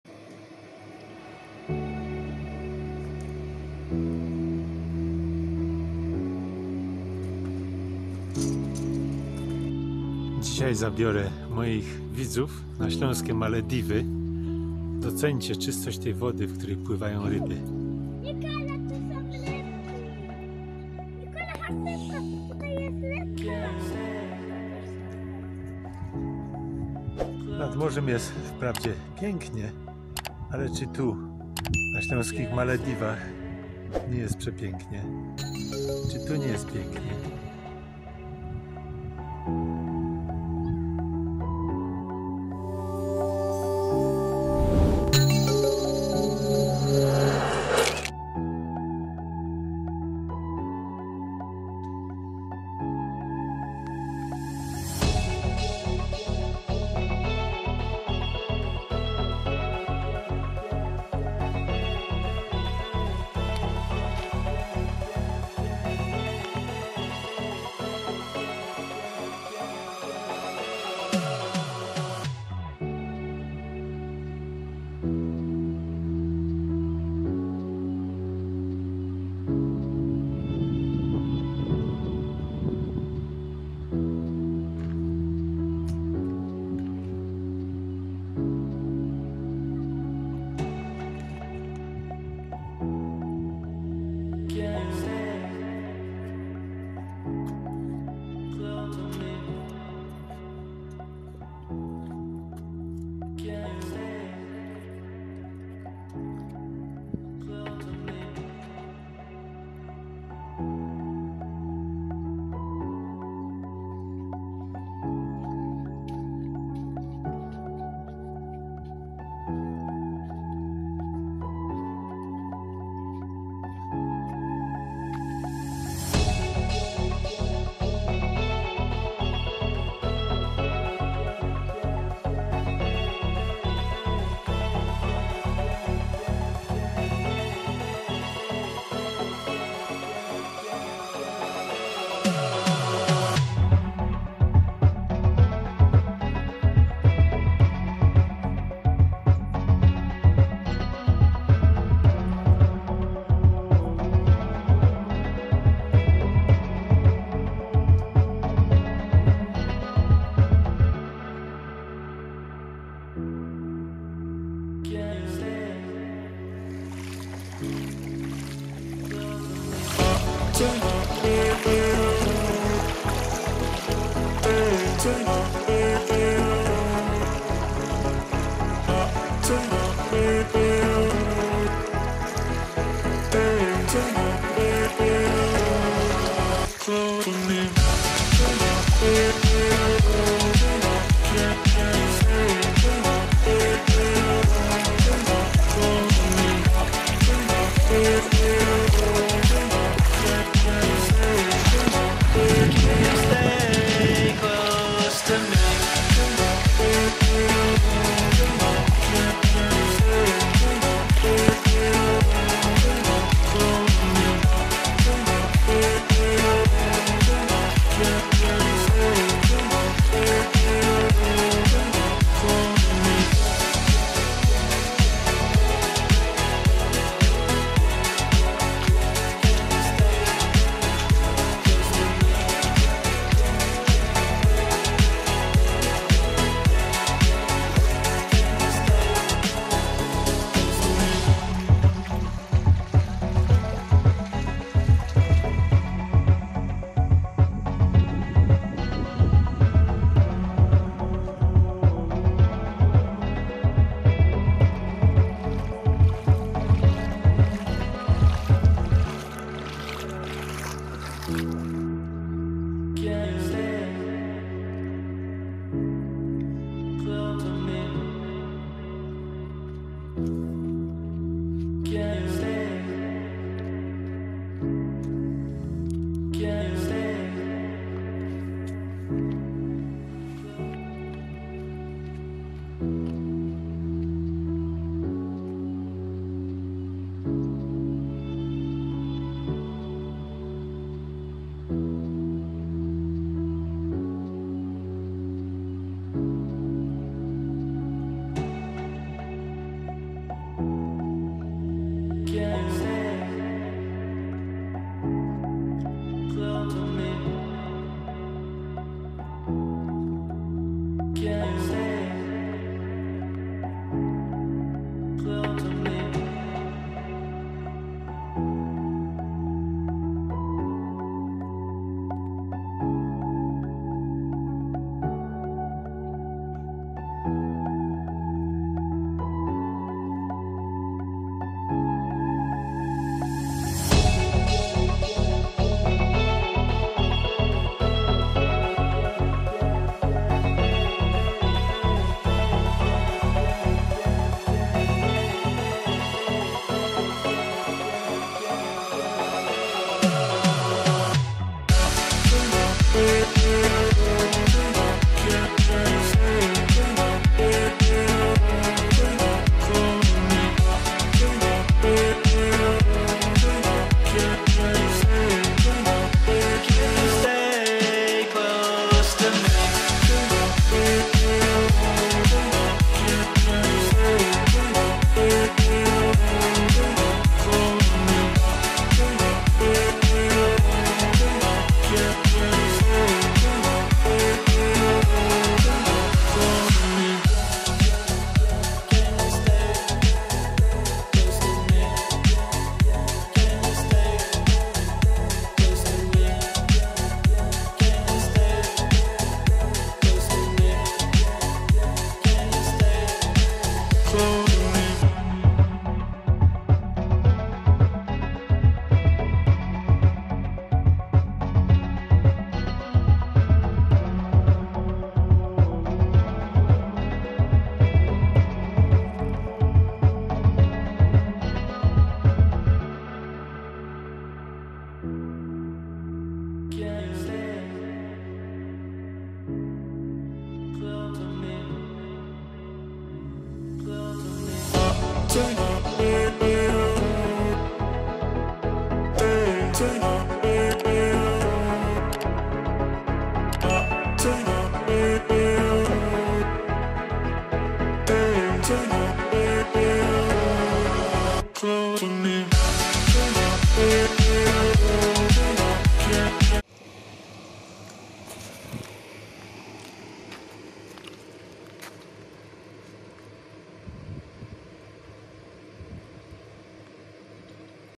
MALEDIWY za 15 zł (parking). Atrakcja turystyczna na Śląsku.
Za jedyna 15 zł można znaleźć się na Malediwach. Jest takie miejsce na Górnym Śląsku...
Gwarantuję że warto odwiedzić!